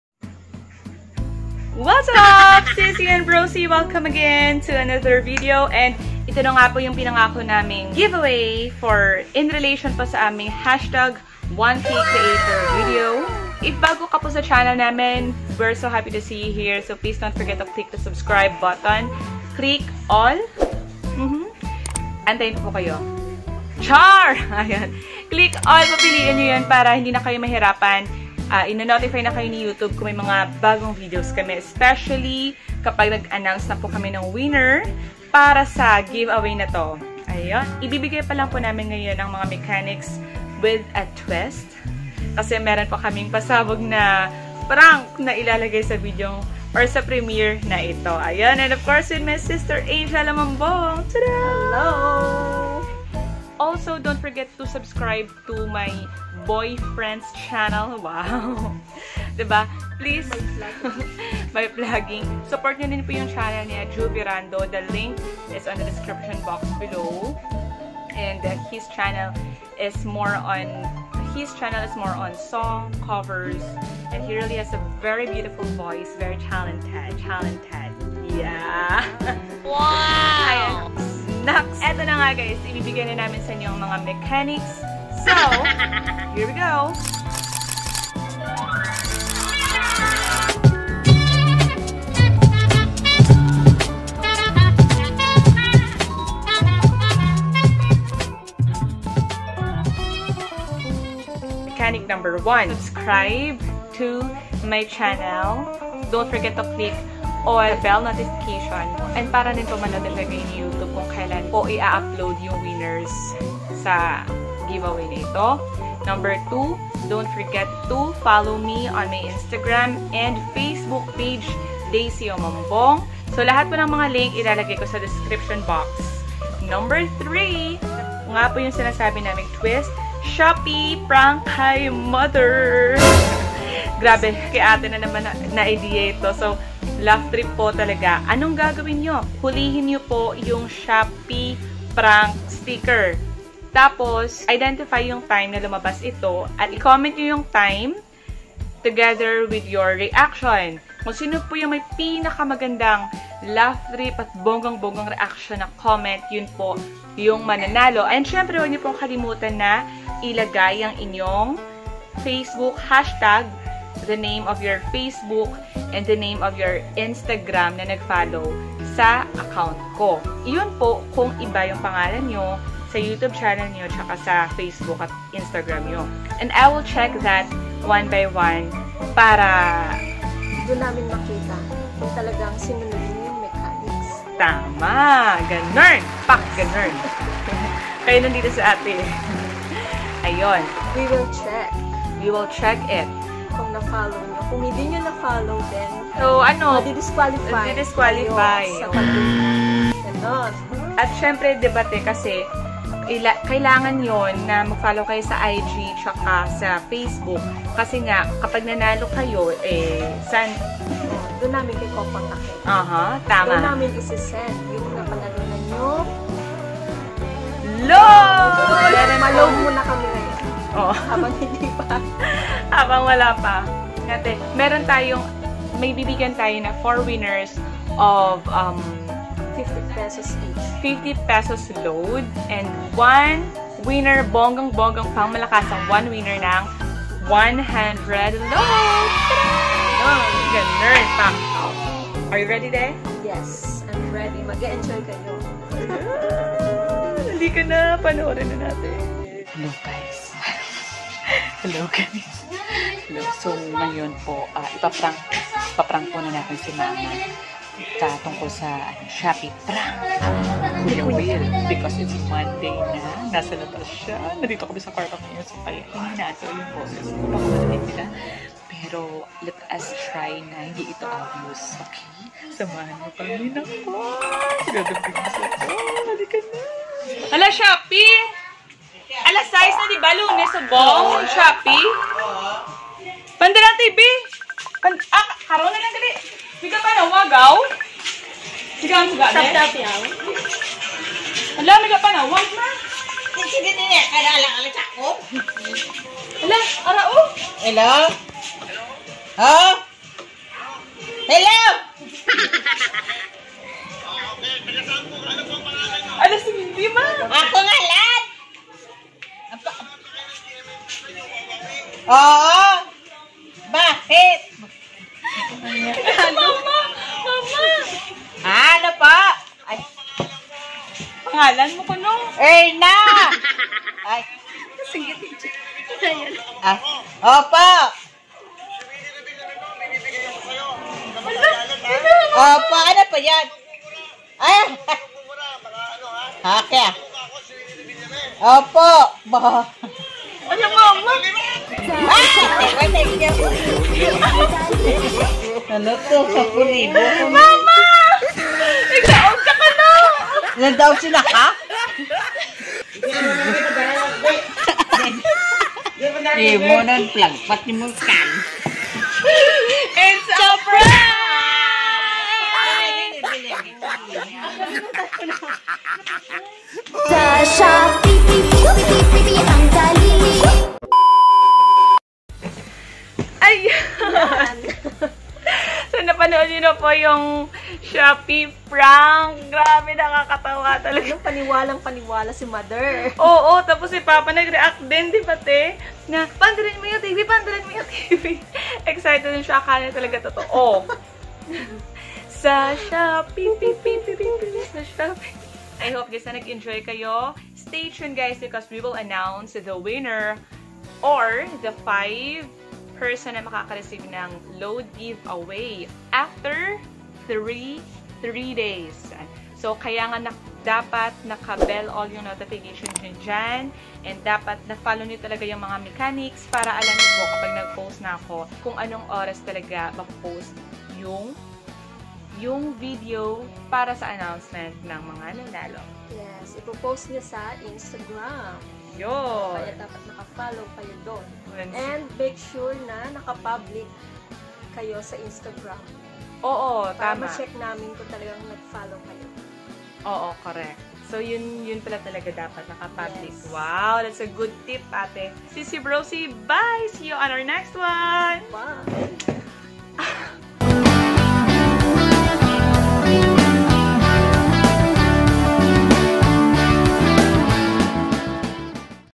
4 WINNERS OF 50 PESOS LOAD, 1 WINNER OF 100 PESOS LOAD(WATCH THIS TO WIN NOW) SHOPEE PRANK kay MAMA
Join us on our first ever load giveaway as our small act of gratitude for your subscription and support.

1 WINNER OF 100 PESOS LOAD
4 WINNERS OF 50 PESOS LOAD

Mananalo kana mag eenjoy kapa sa panunuod. Make sure to FINISH THE VIDEO TO FOLLOW all the Mechanics and get the chance to WIN!

*Socials*

Instagram&Tiktok: @daisyomambong

*Productions*

Video editor: KineMaster
Camera: Oppo F11
Edited by: Daisy Omambong